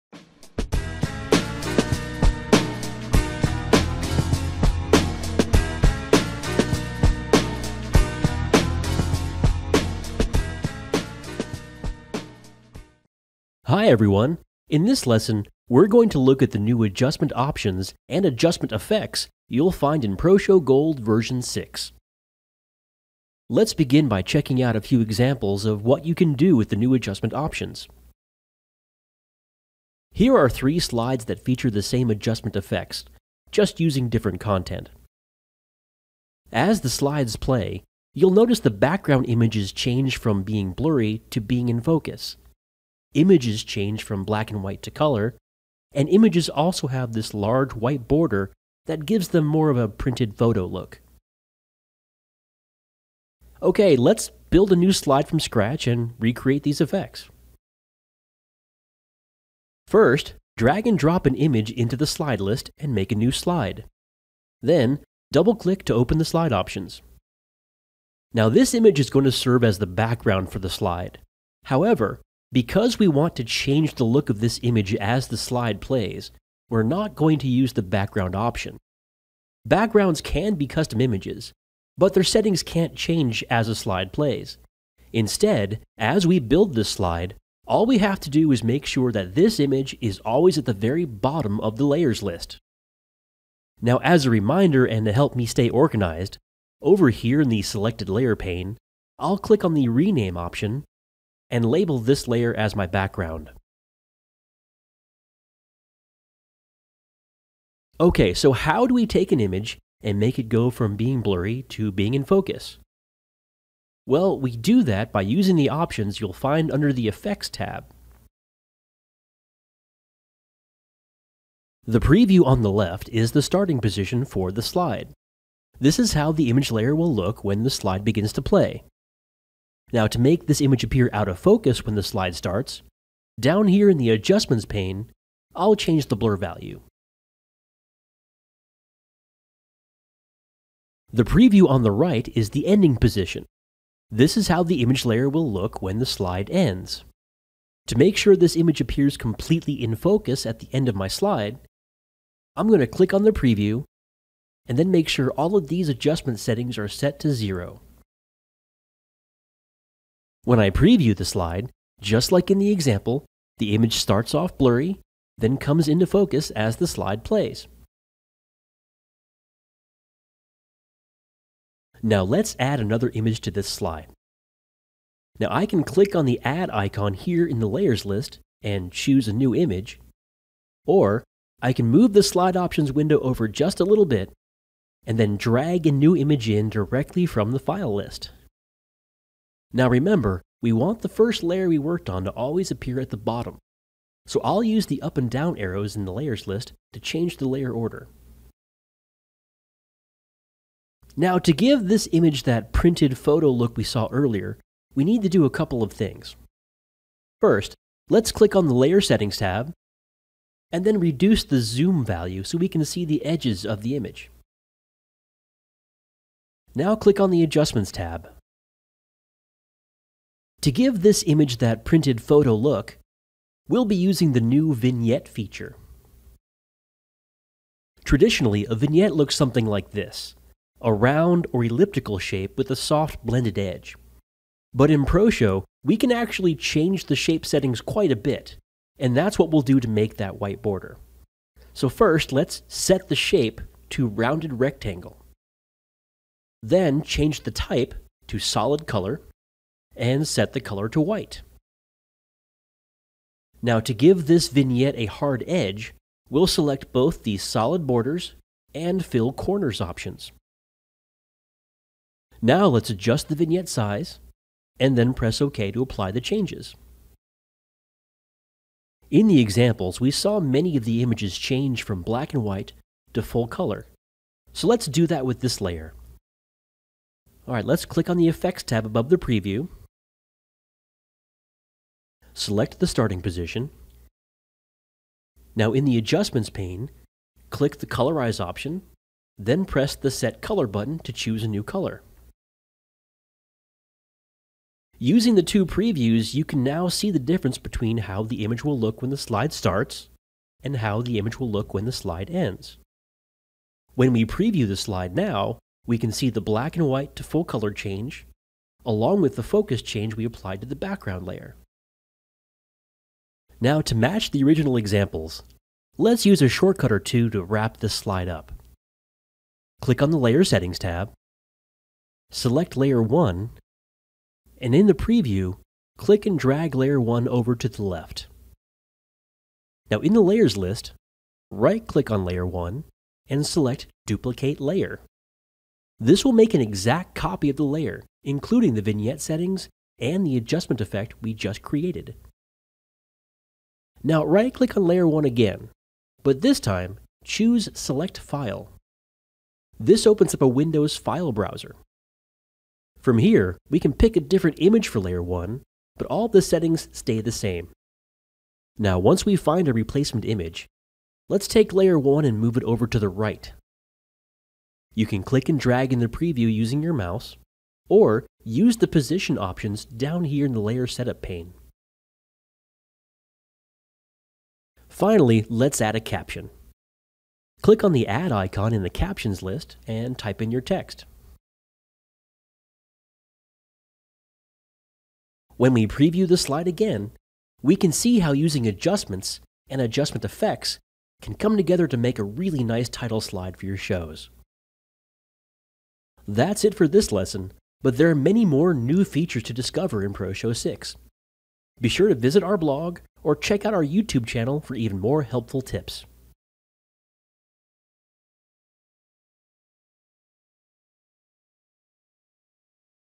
Using Adjustment Effects in ProShow Gold 6
Take a look the new Adjustment options and Adjustment Effects you'll find in ProShow Gold Version 6 and how you can use them creatively in your slideshows.